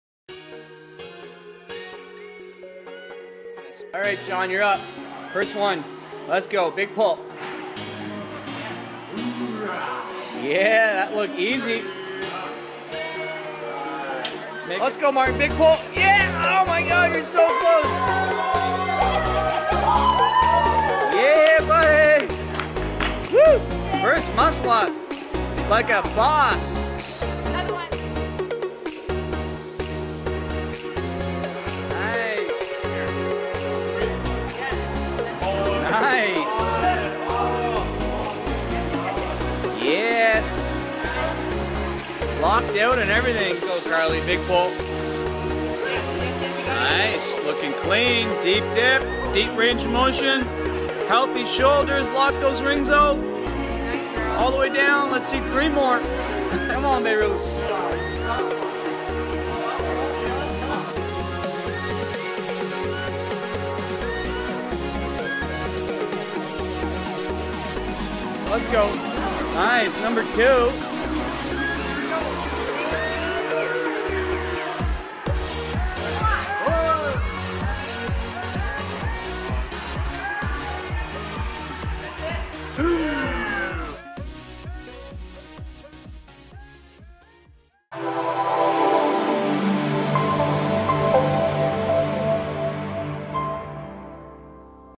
EVERYONE GOT A MUSCLE UP !!
Everyone in rings strength class got their first muscle up, and some a even few more :)

Don't limit yourselves!!